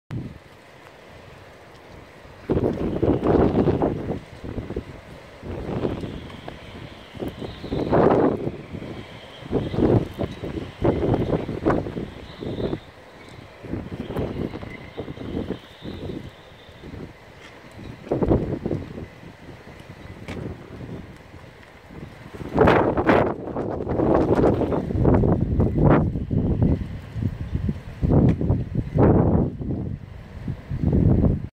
Random Strong Wind and Rain - Newbiggin by the Sea
A video of some random strong wind in Newbiggin by the Sea, Northumberland, UK